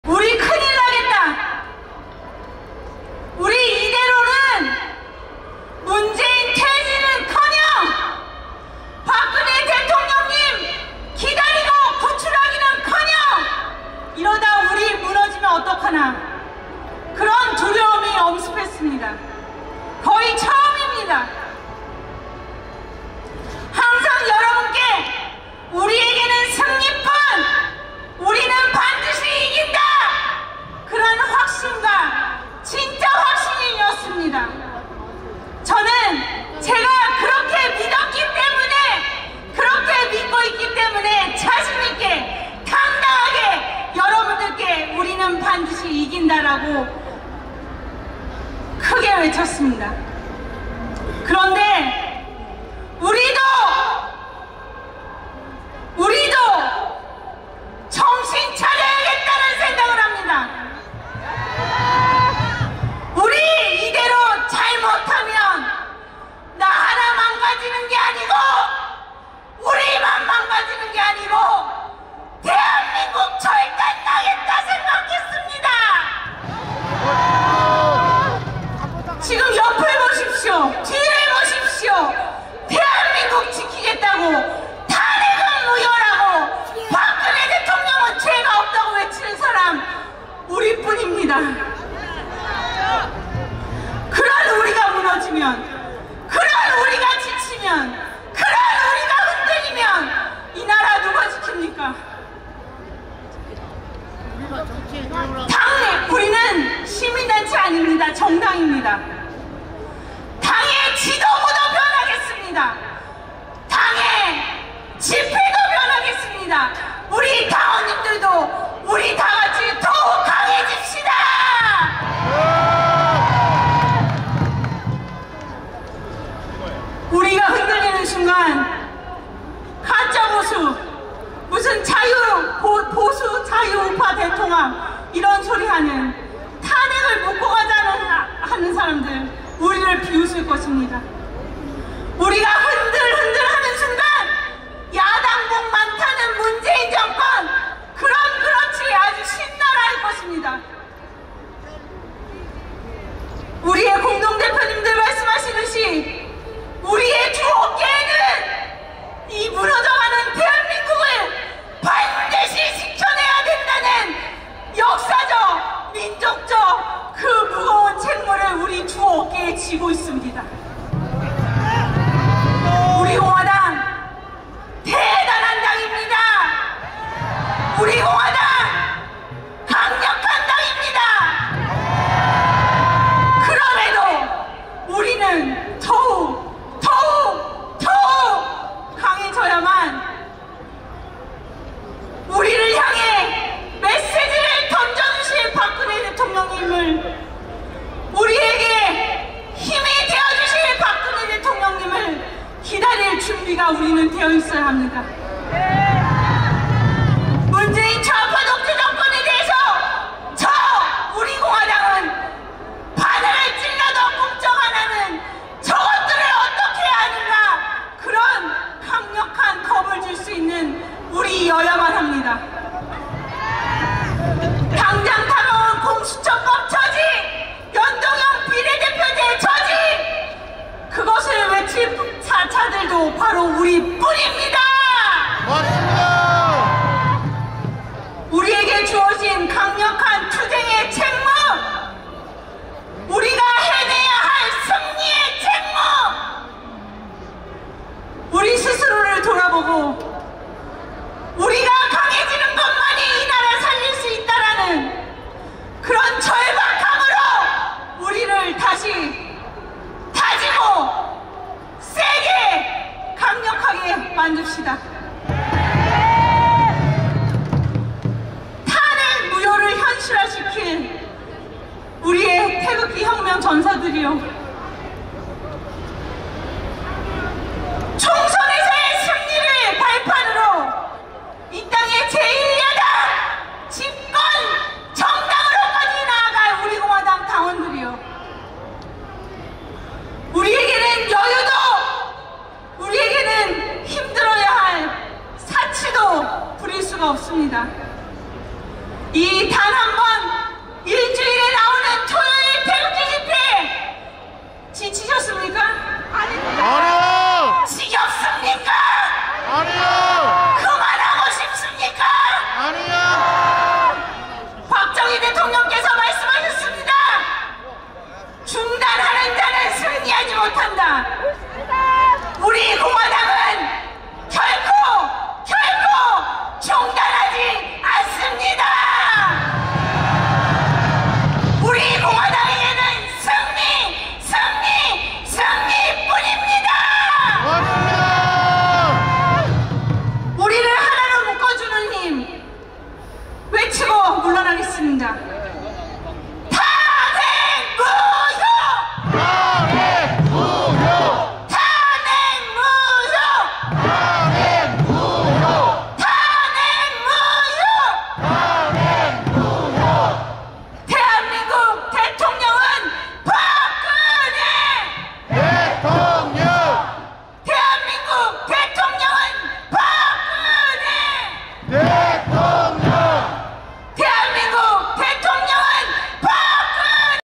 우리공화당 당원분들이라면 한번만 들어주세요 ♡ 일주일에 한번 나오는 집회 지치셨습니까?인지연대변인의 감동 연설 !
출처:TV ba jin 우리공화당 대한민국이 정말 ...
다 망해간다..
마지막 남은 투사 우리공화당 당원분들이여...
추위와 싸우더라도 집회에 참석 합시다.

자유를 외칠날도 얼마 남지 않을꺼 같아
마음이 아픕니다

이번주는 일마치고 집회참석을 하였기에
2부집회때 도착 .

늦을껄 알았지만
아주 중요한 시기에
우리가 무너지면 안된다는 사명감으로
서울로 향했다.서울로 향하는중 인지연 대변인님의
연설이 가슴을 찡하게 했다...

2부집회 세종문화회관앞 도착..
많이 줄어든 당원분들을 보며 나도 마음이 아팠다.
우리대표님들도 얼마나 힘이 빠지실까 하는 생각이 들었다.

나하나쯤이야 하는 마음이
다 무너질 수 있다는 걸

그 와중에도 ... 행진속에 만난 어느.. 나이 많으신
할머니.. 지팡이를 짚고 걸어가신다.
날씨도 추운데...왜 ...?

나라가 다 망해가니..
도저히 집에 계실 수 없으셨는 거 같다..
작은 힘이라도  더 하기위해 나오셨을텐데
사람은 역시 정신력으로
움직이게 하는 것 같다.

귀찮아... 힘들어...
추워서 못가겠다...하는 순간 무너진다.
우리나라는... 먹혀버릴 것이다.

'나부터라는 '마음으로 다시
움직여야 할때라고 본다.

지금은 정신을 바짝 차릴 때!!

자유민주주의를 외칠 수 있을 때
우리 집회에 참석하여 외칩시다!

자유는 그냥 얻어지는것이 아닙니다 .